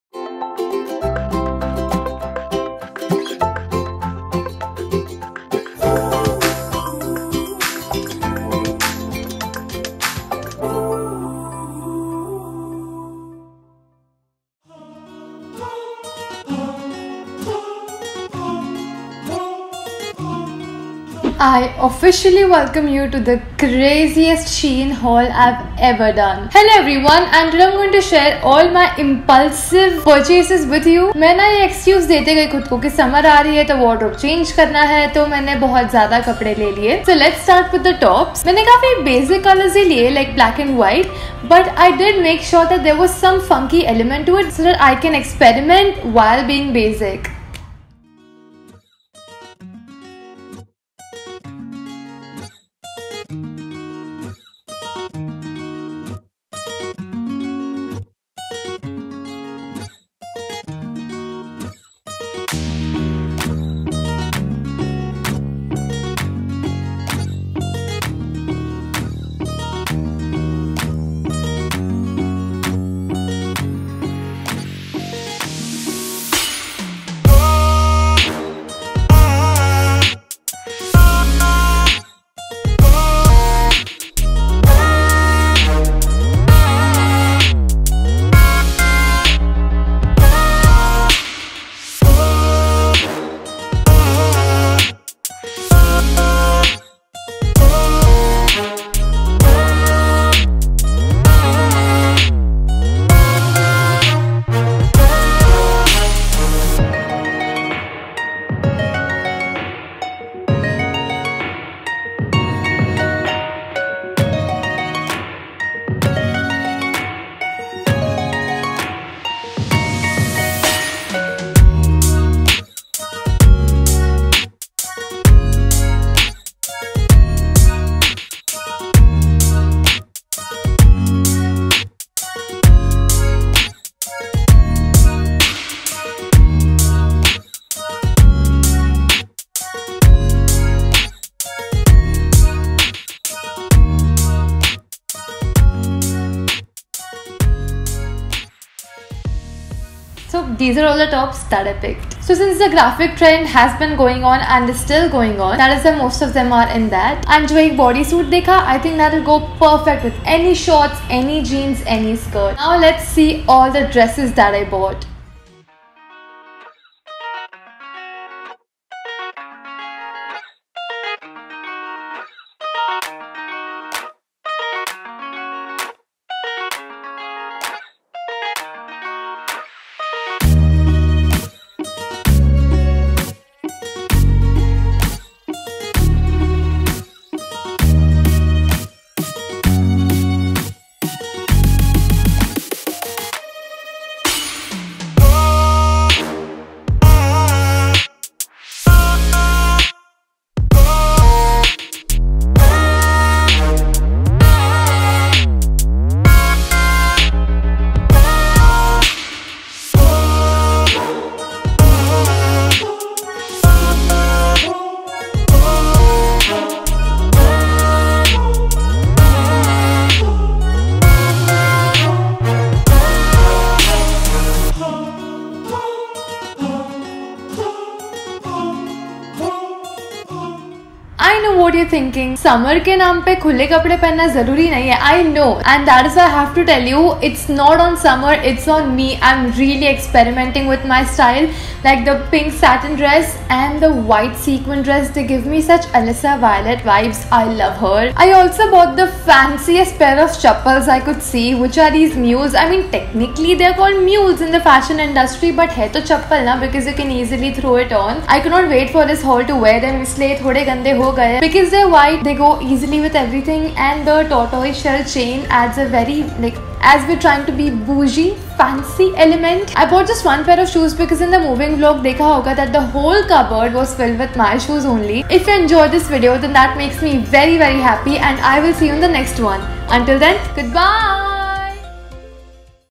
*HUGE* SHEIN Haul! Latest Summer 2019 Fashion Edition | Heli Ved
If you'd like to try any of these outfits :) -

Tortoiseshell Decor Point Toe Flat Mules

Neon Lime Bodycon Cami Dress

Textured Floral Slip Dress

Open Back White Glitter Sequin Halter Dress

White Solid Surplice Neckline Bodysuit

Surplice Ruched Asymmetrical Hem Cami Dress

Solid Cami Dress

Lace Up Backless Satin Bodycon Dress

Popcorn Tee

Pop Art Tee

Newspaper Tee

Black and Yellow Figure Print Tee

White Graphic Tee

Love,
Heli ;)